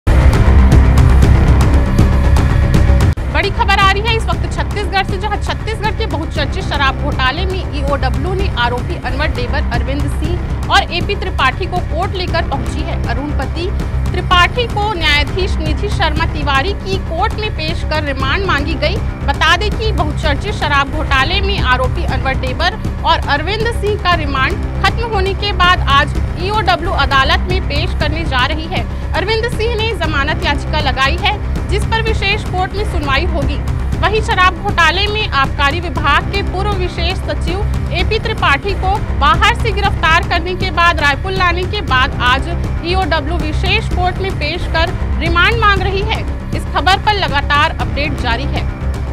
Breaking News : कोर्ट में पेश हुआ अनवर ढेबर और अरविंद सिंह के साथ एपी त्रिपाठी
Breaking News AP Tripathi appeared in court along with Anwar Dhebar and Arvind Singh